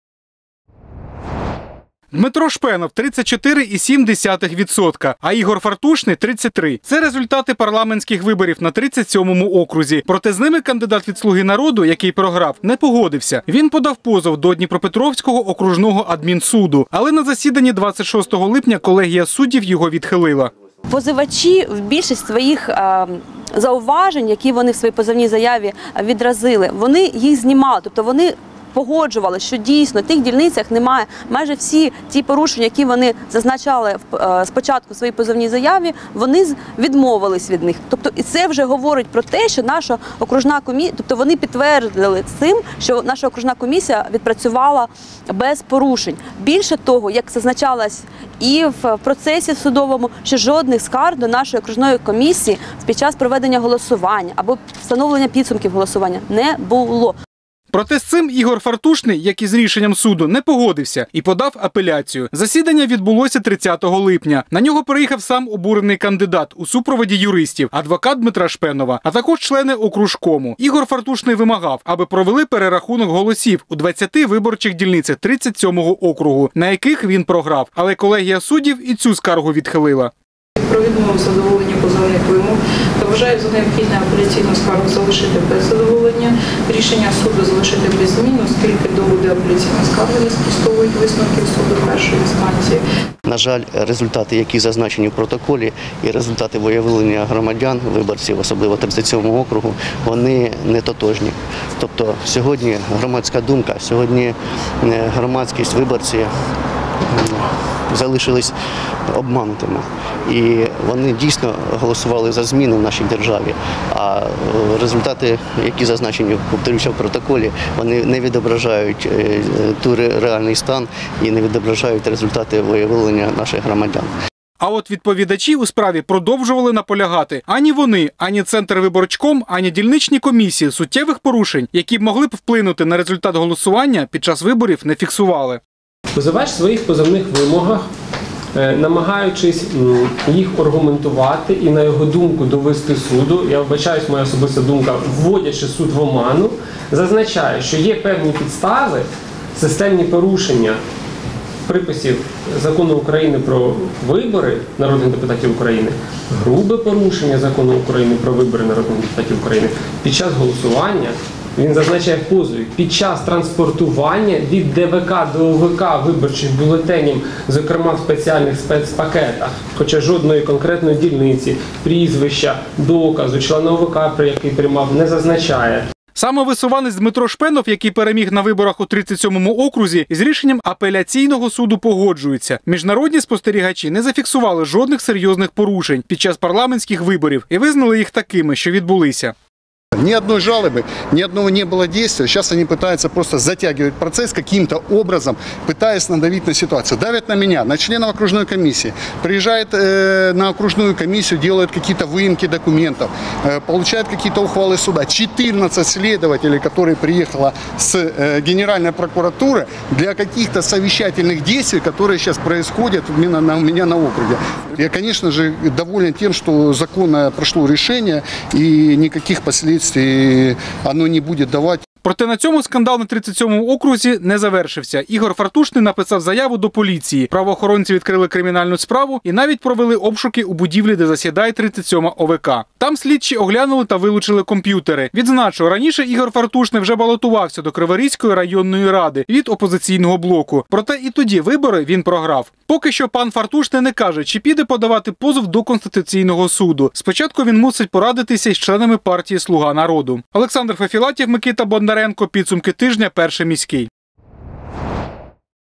Справа 37-го округу
Перерахунку голосів по 37-му округу не буде. Два оскарження від кандидата «Слуги народу» Ігоря Фартушного – до Дніпропетровського окружного адмінсуду та згодом до Апеляційного – залишились без задоволення. Свою поразку на виборах до Верховної Ради самовисуванцю Дмитру Шпенову визнавати не хотів, тож у судовому порядку намагався опротестувати результати волевиявлення. Проте всі його доводи суд не взяв до уваги. Як розгортались події стосовно спроби переглянути голосування на 37-му окрузі, дивіться в матеріалі наших кореспондентів.